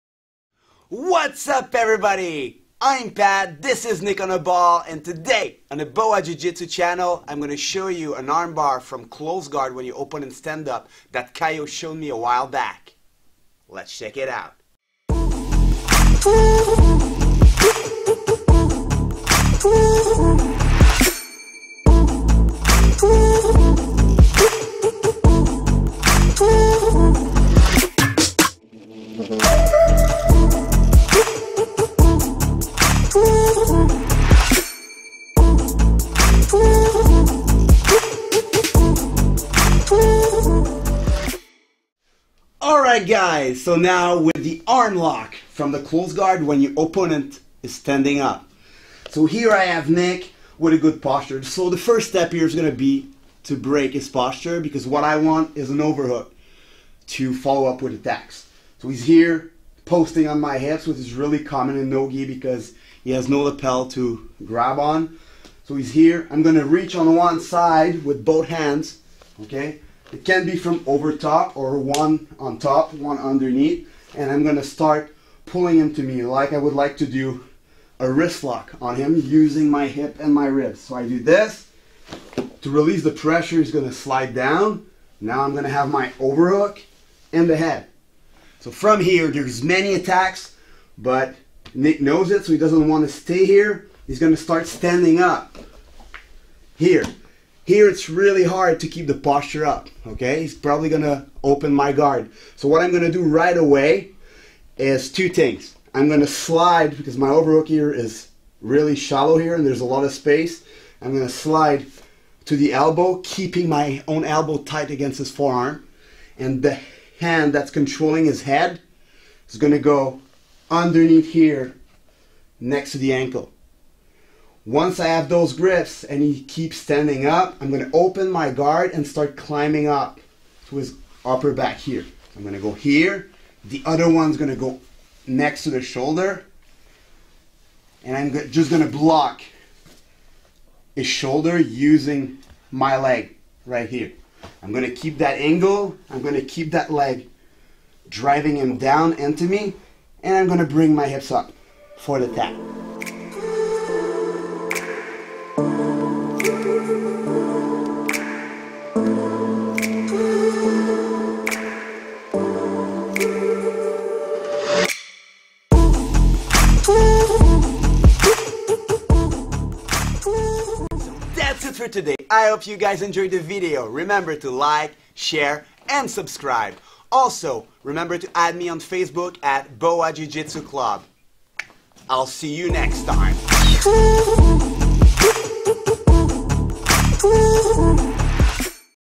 How to Armlock From Closed Guard - When your opponent is standing up
How to armlock / armbar your opponent when he is standing up in closed guard.

Today, I am teaching you an armlock setup that I learn from my professor, Caio Terra. It is a very useful tool when your opponent is standing up from your closed guard. It works well with and without the gi. OSU